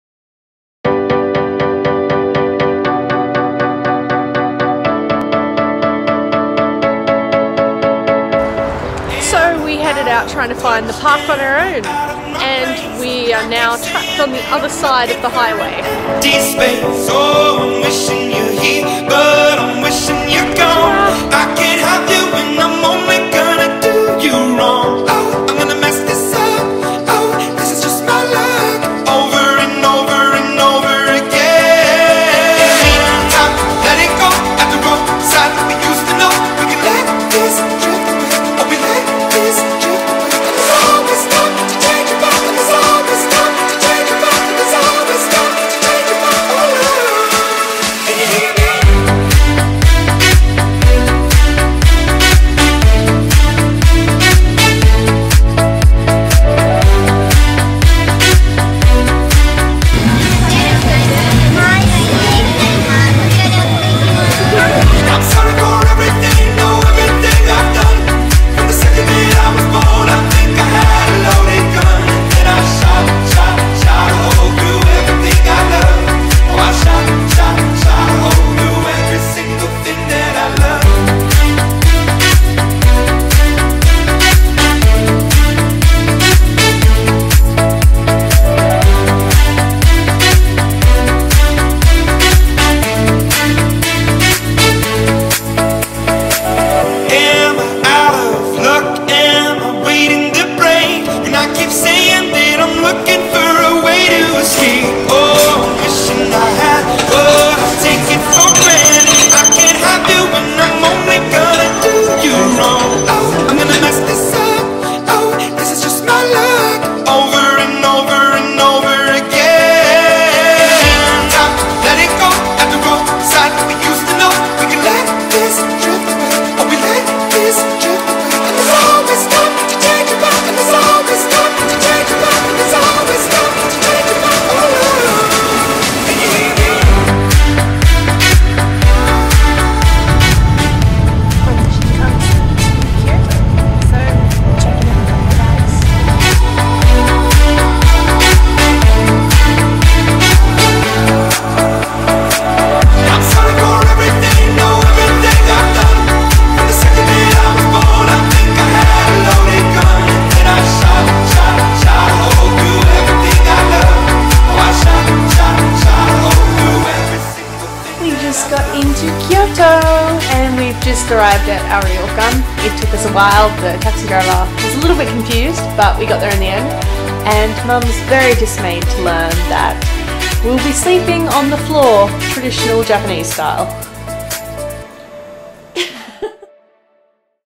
EVERYTHING'S BIGGER IN HARAJUKU! - Surviving Japan 101
SURVIVING JAPAN 101 - How to get by in Japan with nothing but a questionable map, a good sense of direction and a few years of high school Japanese...

Day 4 is all about big, as we visit Tokyo's biggest park, Yoyogi Park, the famous Shibuya crossing, and of course Harajuku.  Day 5 takes us on a shinkansen trip to our next stop, Kyoto!